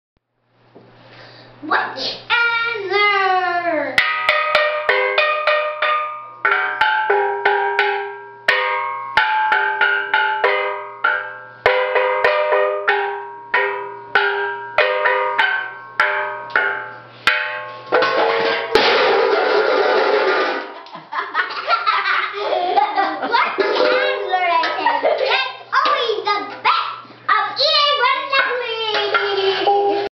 Steel Drums - funny brother
Totally not planned... I like how the steel drum shakes the camera stand.